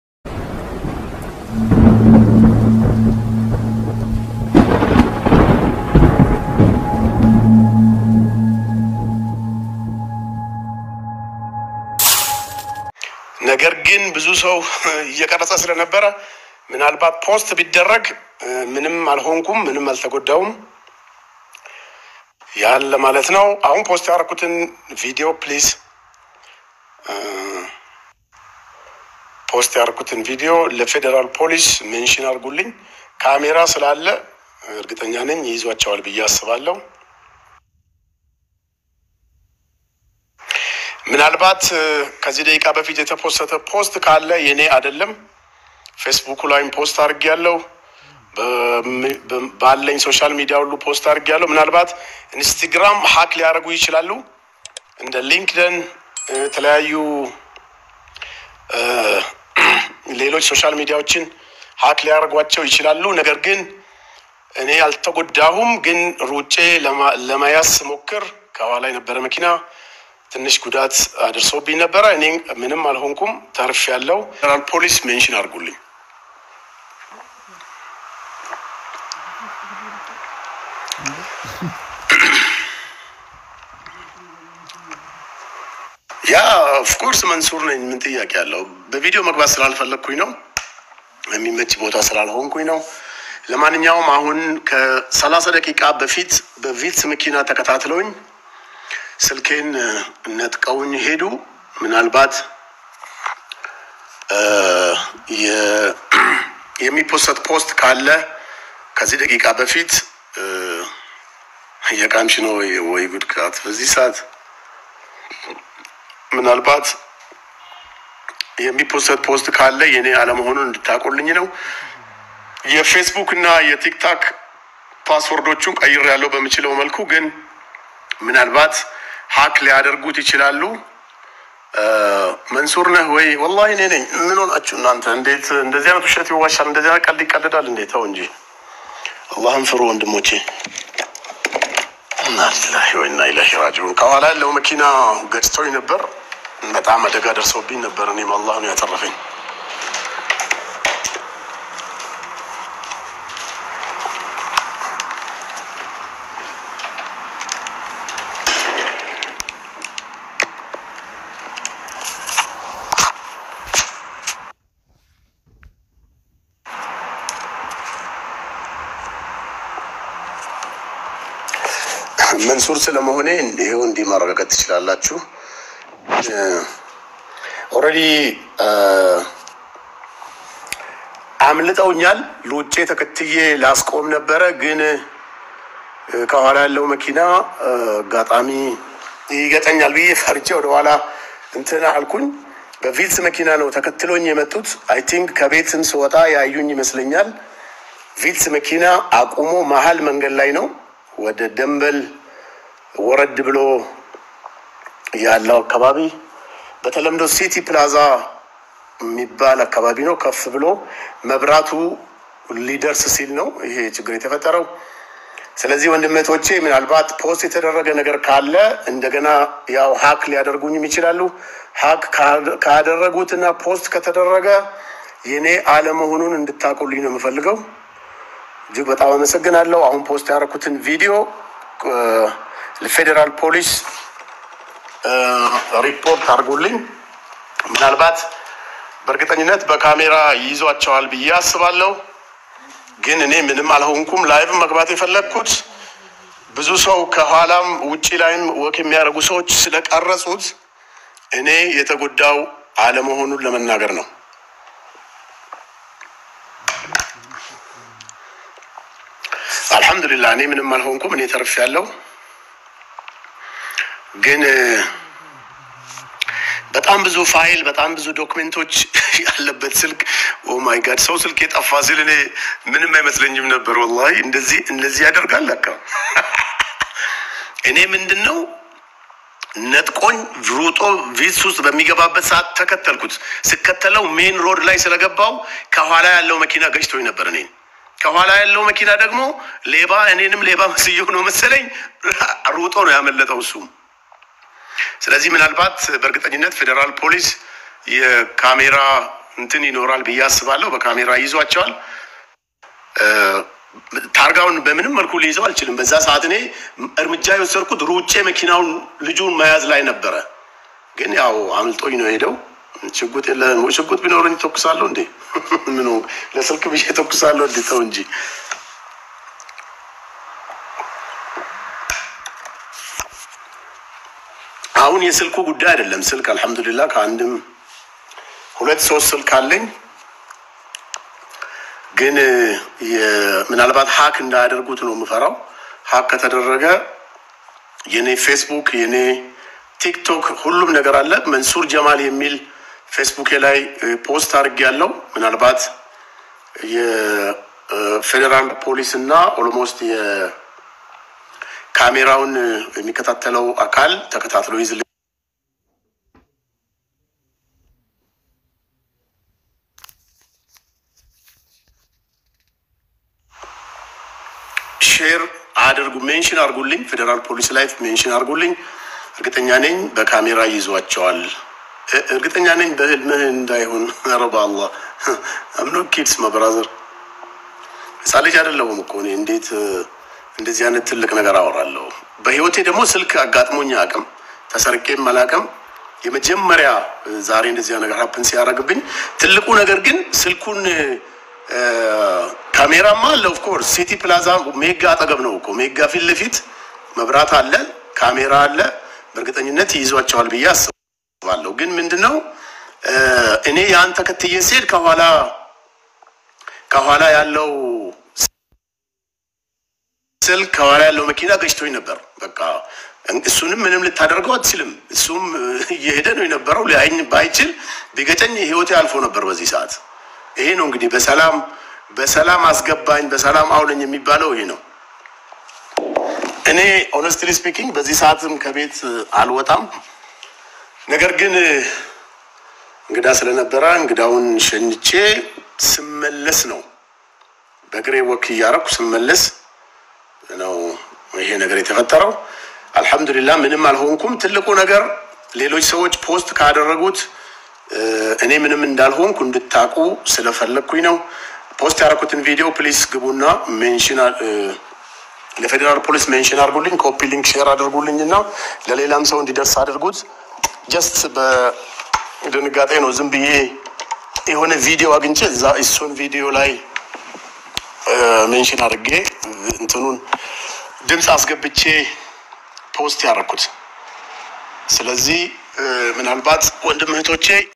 መንሱር ጀማል ከሌቦች ጋር ተናነቀ
መንሱር ስልኩ ተበላ